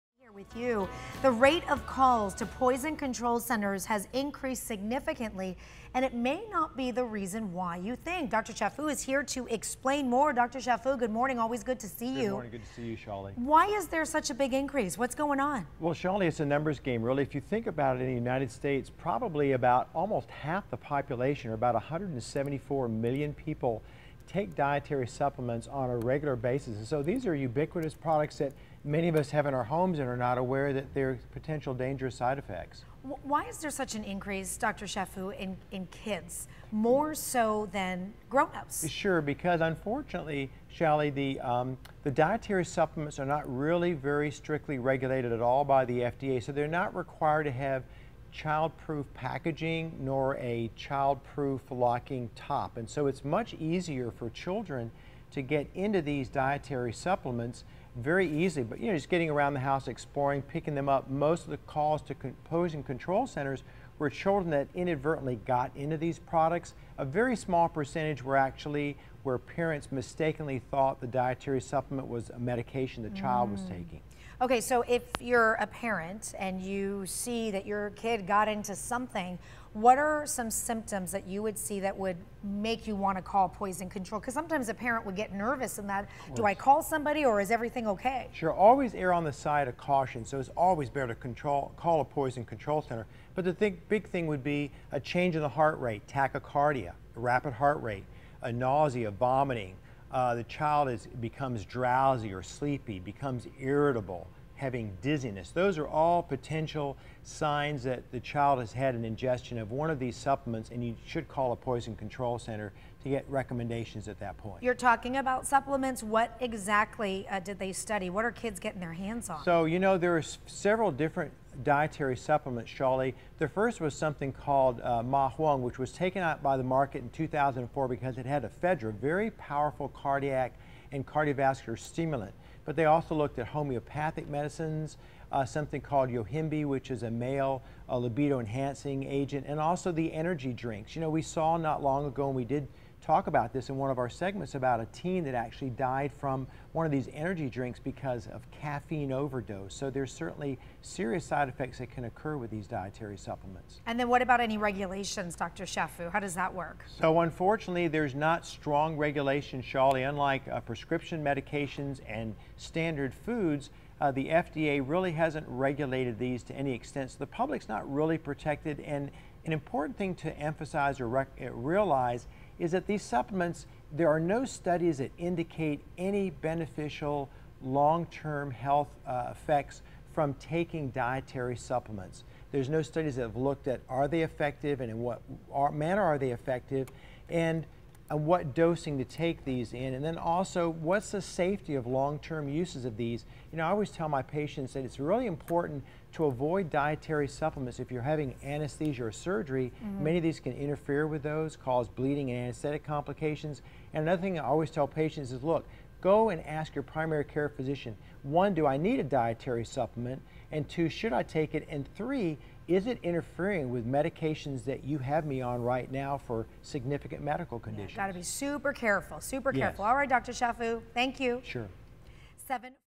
Fox5 - Supplements Cause Increase Calls to Poison Control - July 25, 2017
Dr Richard Chaffoo talks with Shally Zomorodi from Fox 5 San Diego about the reasons why there have been an increase in calls to poison control, especially for kids, because of "health" supplements